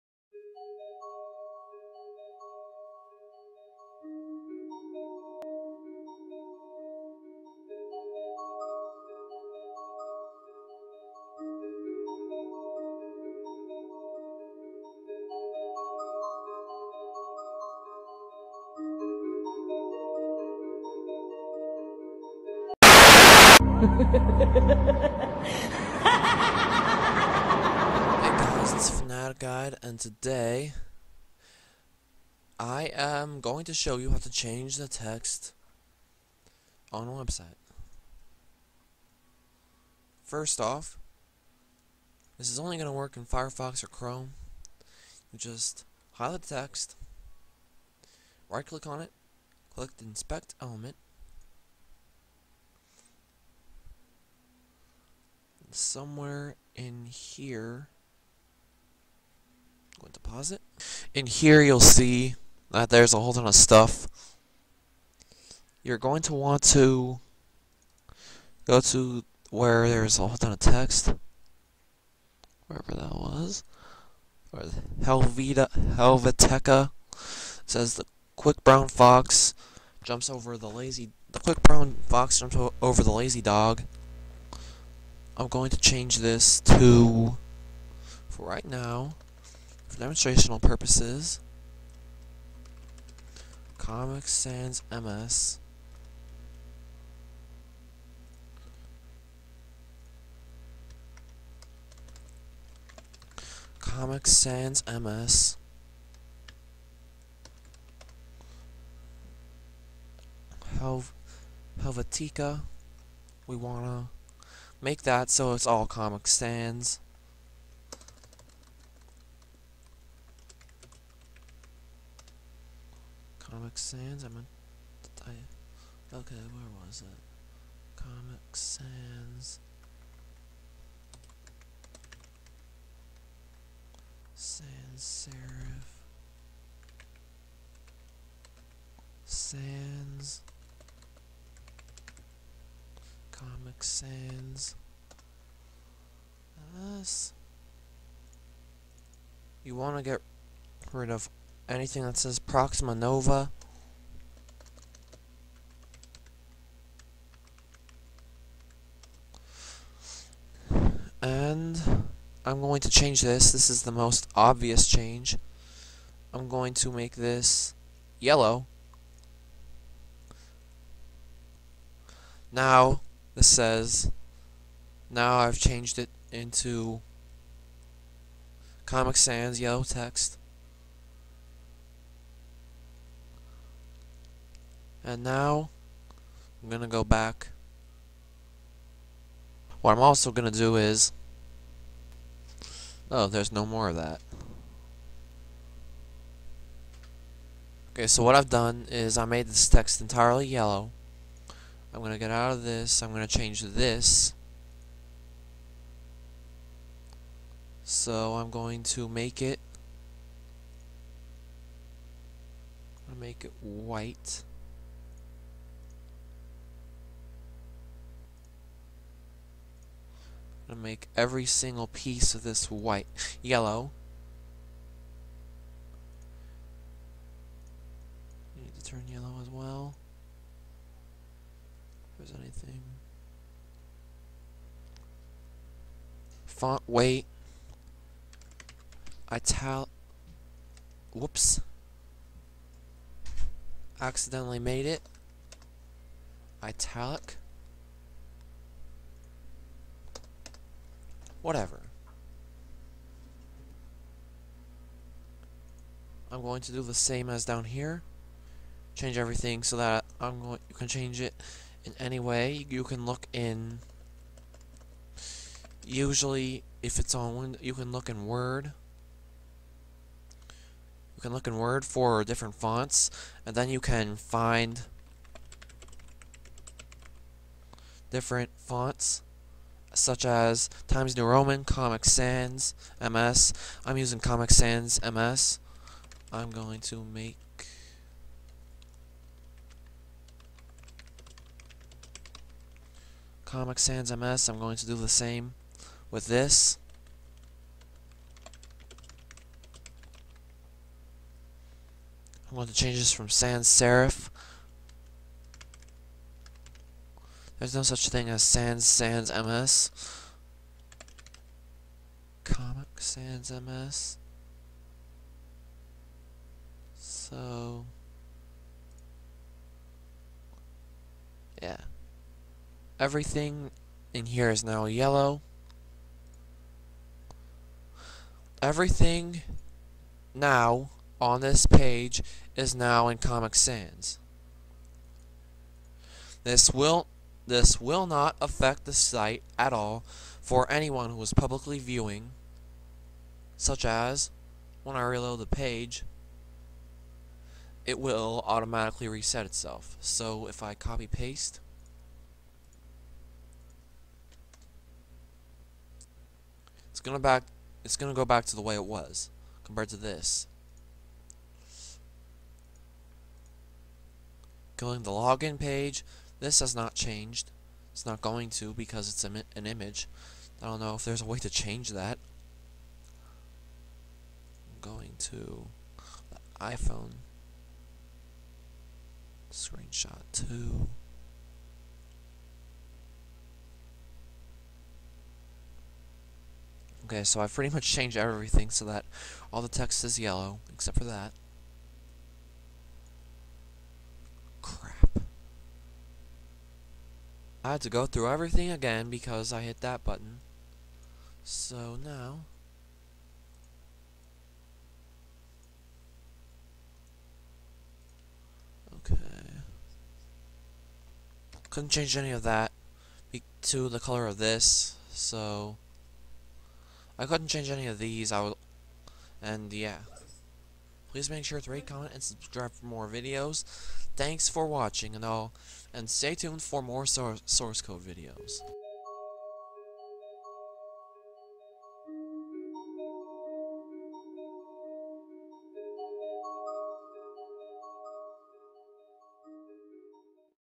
How to Change the Font and Color of Text On an Internet Page via Inspect Element and Source Code
Warning, if the page is reloaded or if you go to another page, the changes made will not be applied. Enjoy the video.